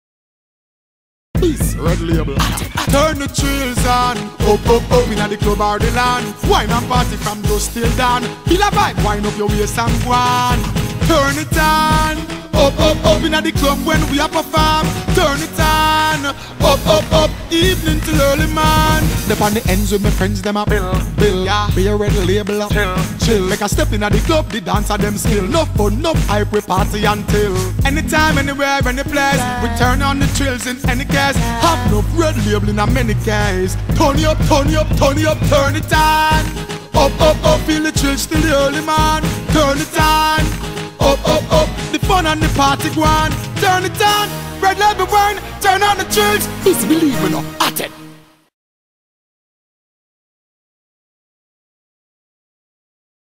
Busy Signal Red Label Wine TV Commercial
Juke Boxx recording artiste Busy Signal, makes an appearance in the TV commercial for Red Label Wine